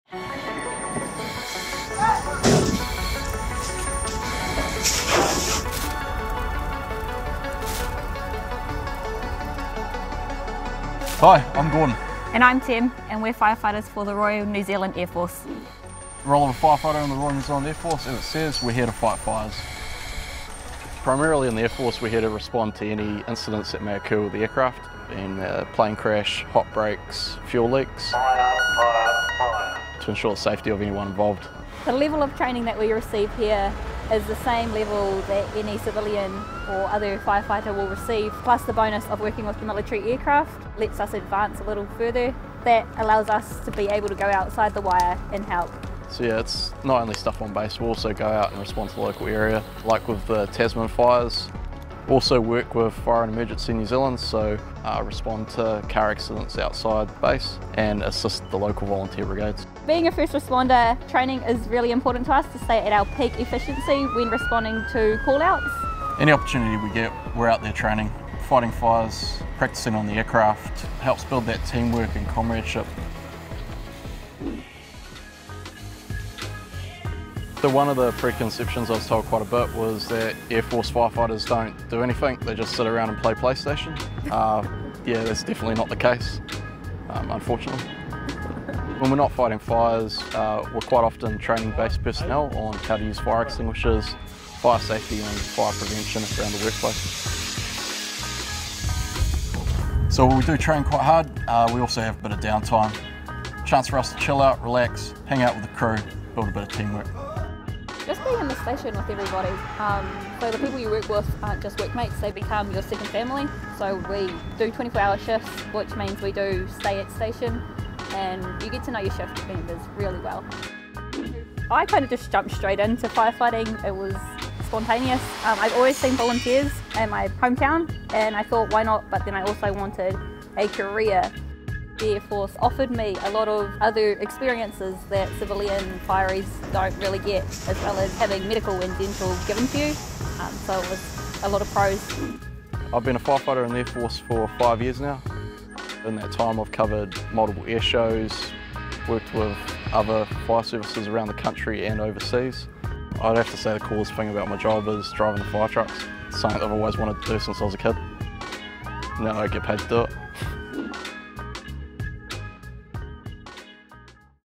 Meet: Tem & Gordon - Air Force Firefighters
Meet Tem and Gordon they are firefighters in our Air Force. Check out what they get up to day-to-day.